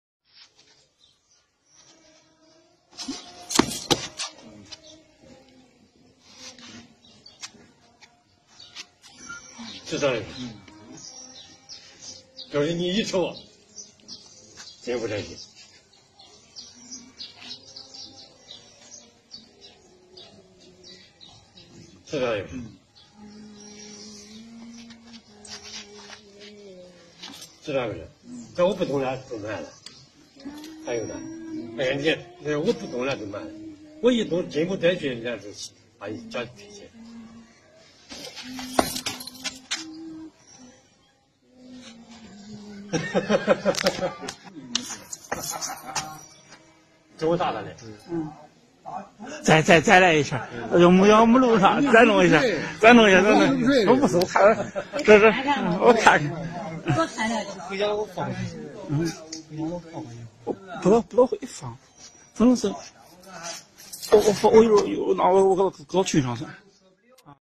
Zhaobao Taijiquan - Zhang Yufeng - tuishou
Zhaobao taiji teacher Zhang Yufeng pushing hands with student